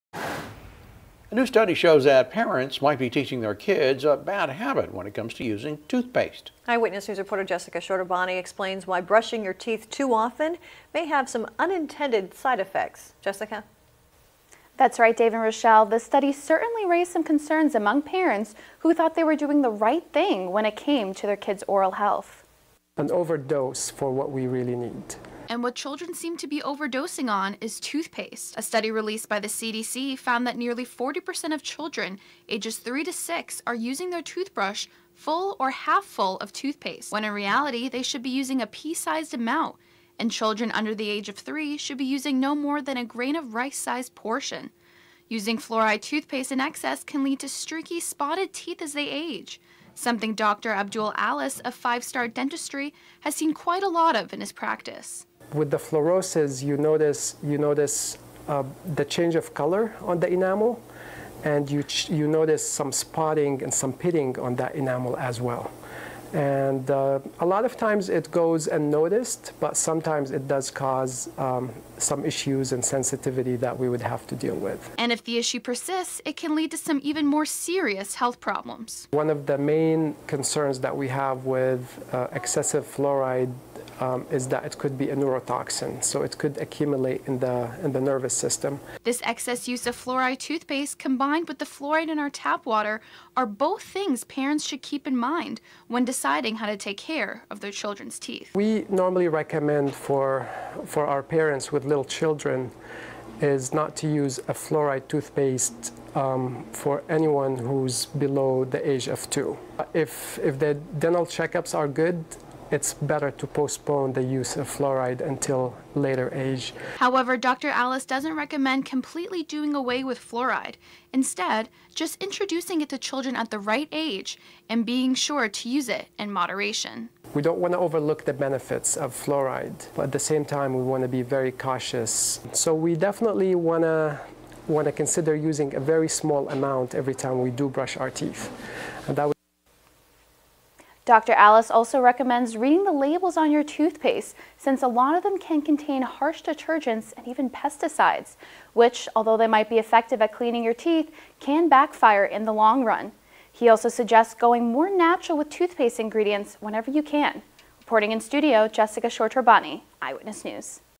WHAT IS A SAFE AMOUNT OF FLUORIDE TOOTHPASTE FOR OUR DAILY USE?
Five star dentistry Bakersfield California local news interview KBAK EYEWITNESS NEWS FOX58.BAKERSFIELDNOW.com

You will be surprised how much less toothpaste we should be using and teaching our kids to use and the side effics that could be caused by using fluoride toothpaste.